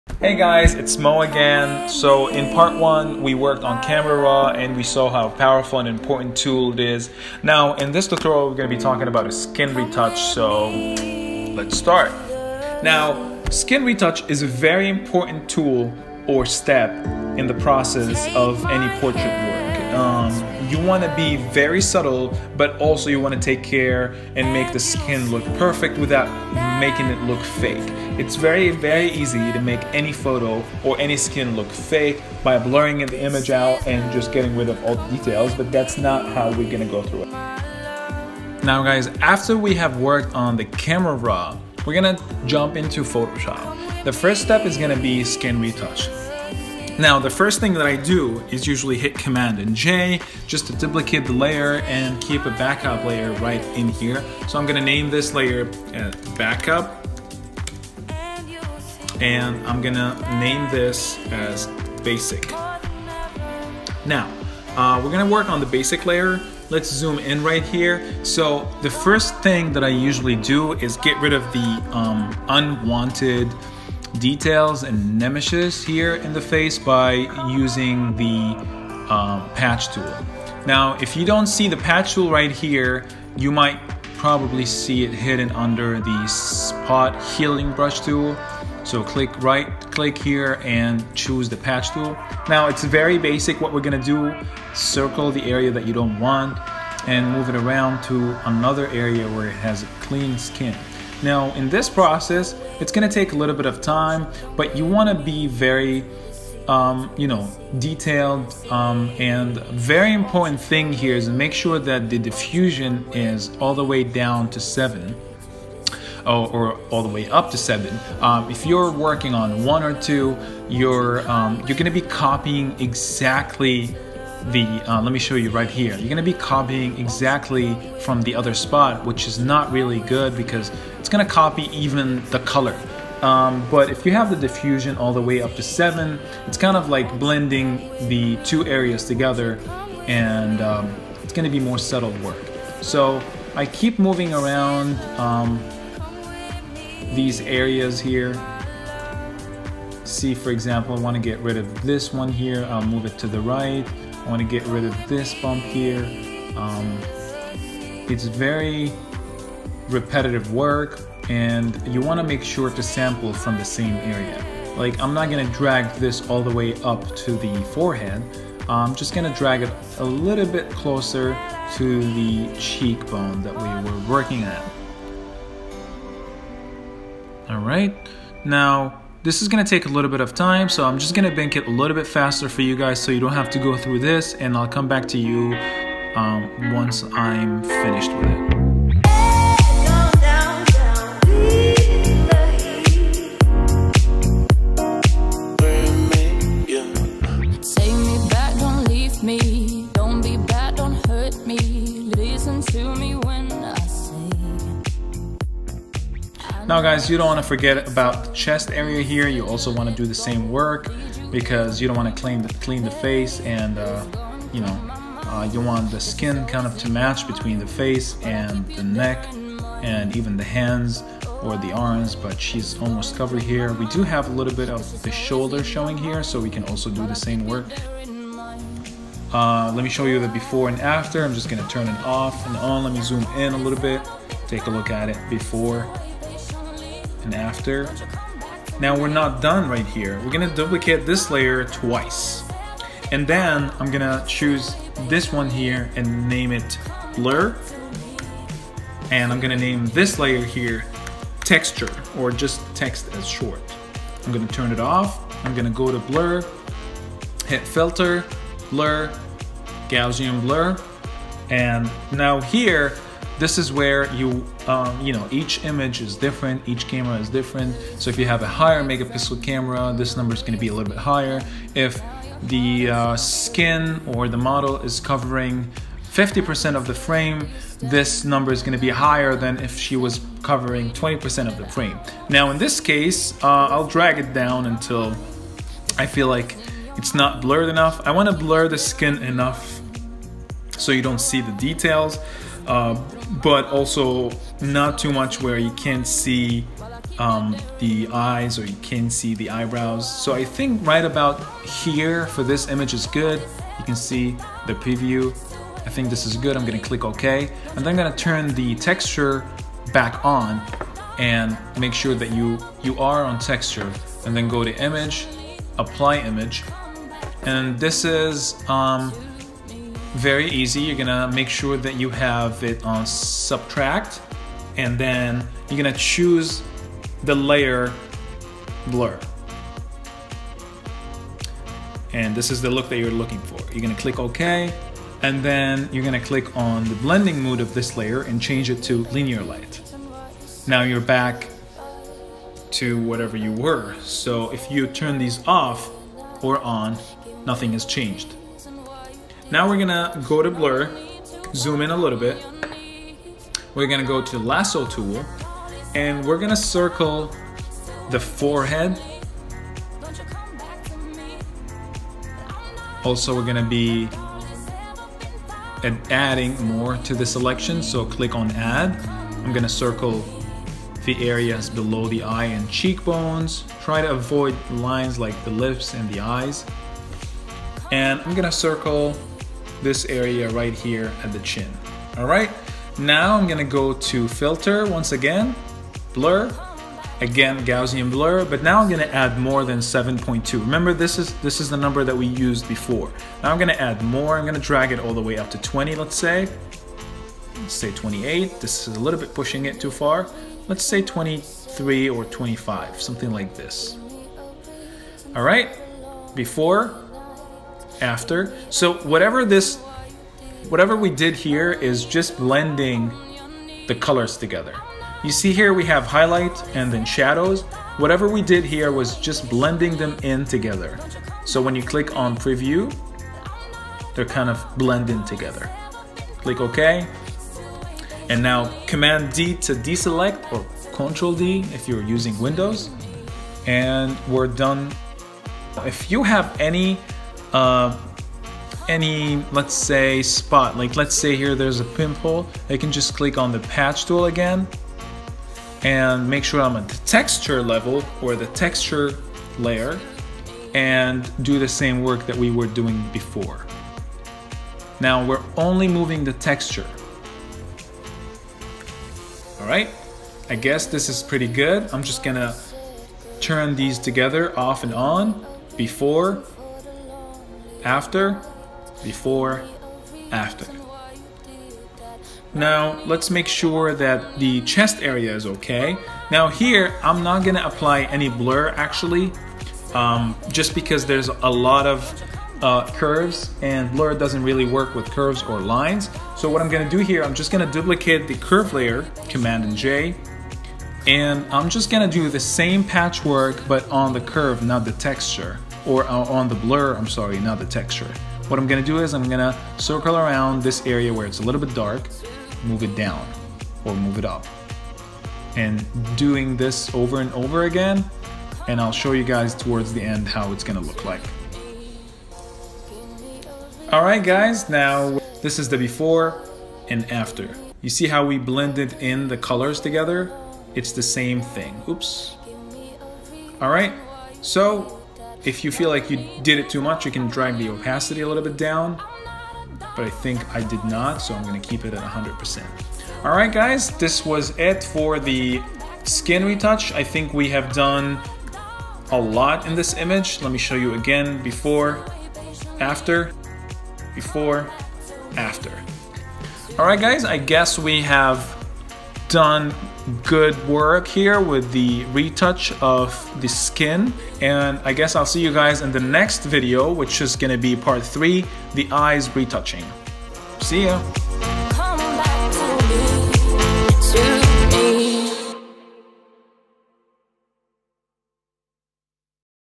How To Retouch Skin in Photoshop | Professional Skin Retouching | Photoshop Tutorial
This Tutorial is Part 2 of the 6. In Part 1 we worked on Camera Raw Adjustment. Make sure to watch that. Now skin retouching is the next step. I will go through my method of frequency sepqration trying to make the skin clean but realistic.